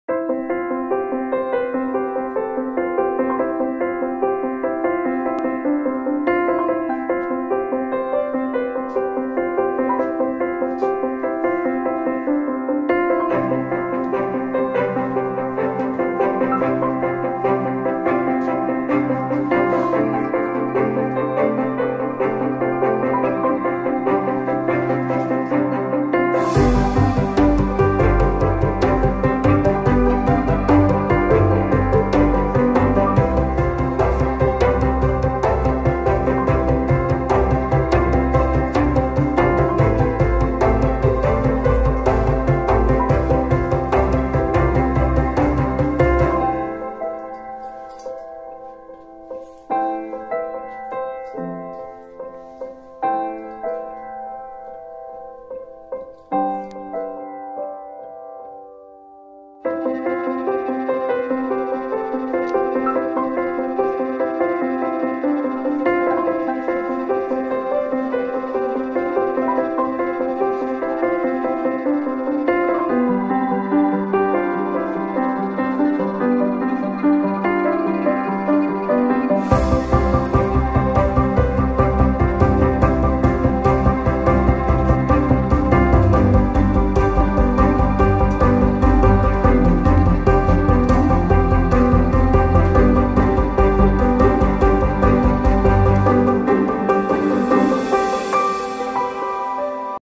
Rustic house!!Nice views,restaured & good decorated!💞💫💙
You can enjoy the inside of a typical rustic house in Catalonia; Oldest ,about more than 100 years,restored and decorated with a new style ,preserving it's structure and some of its antique pieces and objects.Life style & good taste!!Really nice!warm environment💞

Dedicated to @ennaaliascasals7570